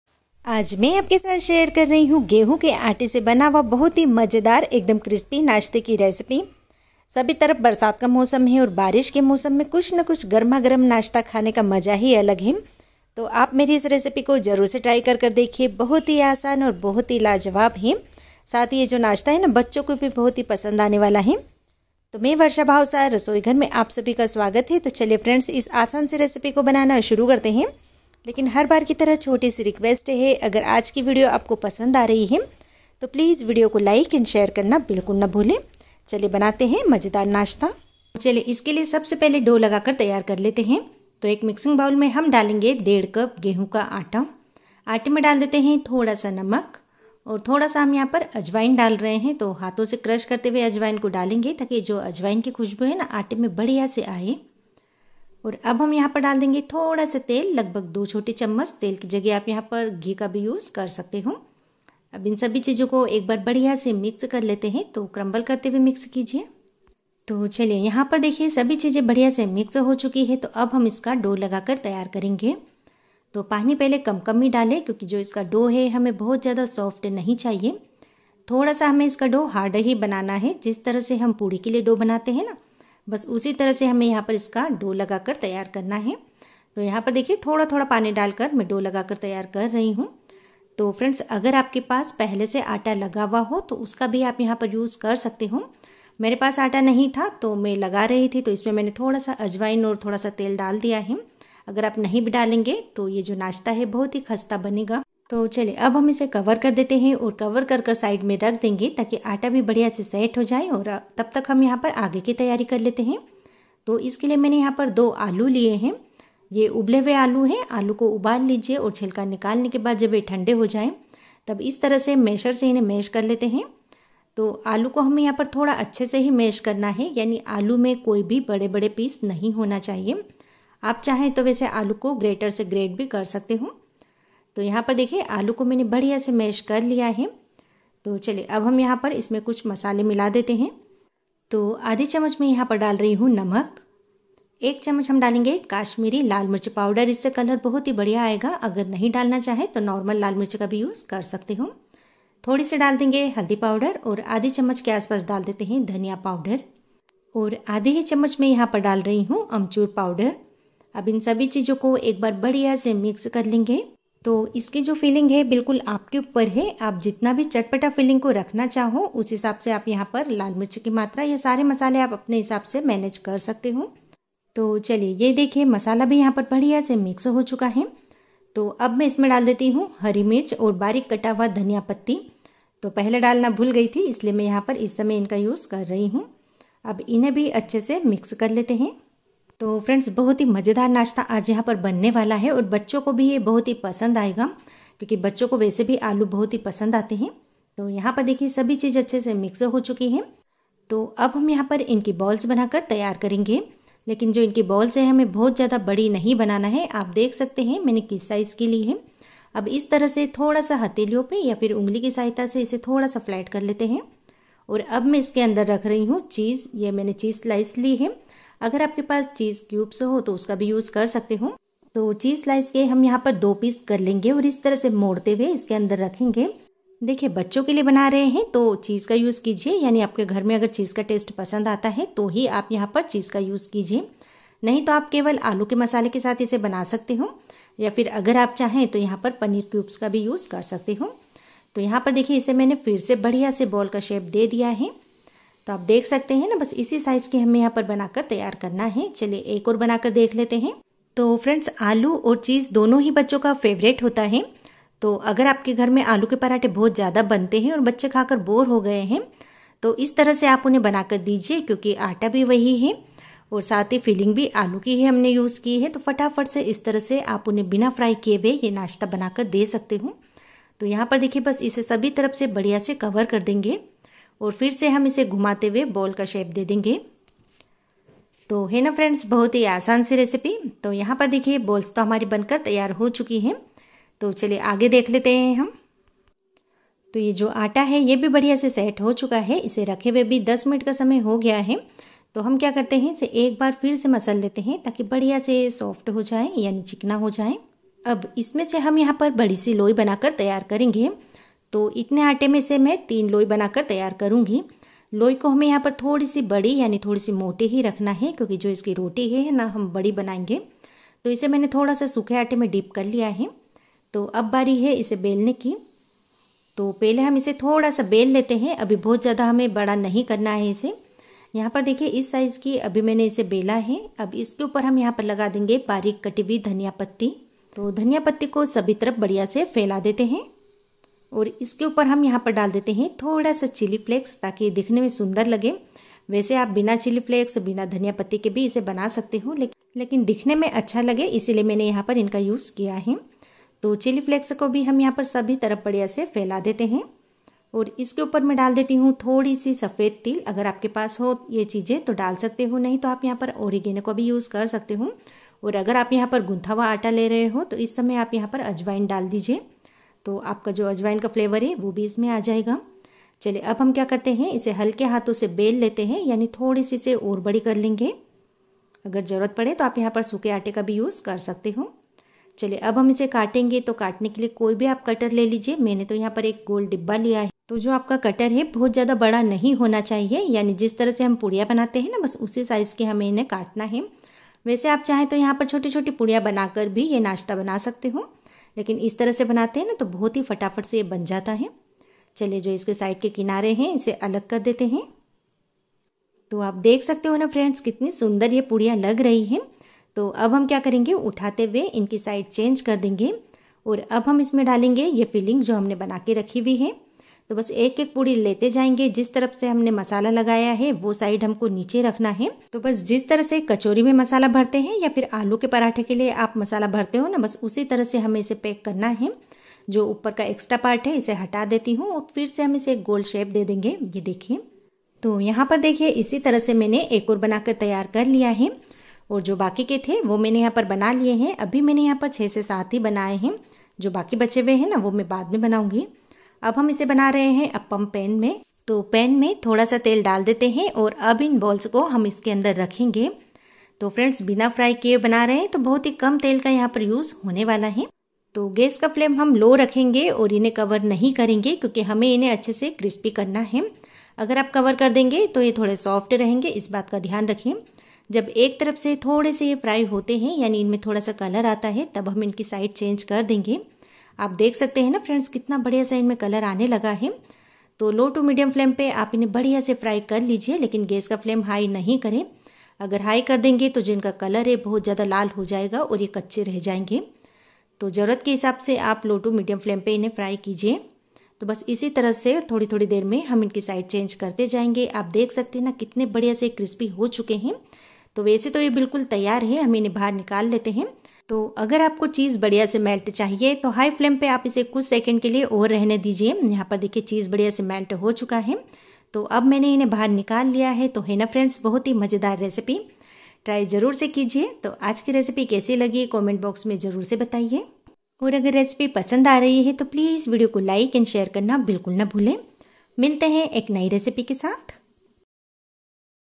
बिना फ्राई किए 1 कप गेंहू के आटे से बनाए गरमागरम मज़ेदार नाश्ता जो भी खायेगा रेसिपी जरूर पूछेगा
Aate ka nashta, gehu je aate ka nashta, Rasoi Ghar, Bina fry kiye Aate ka nashta, Appe, Appam recipe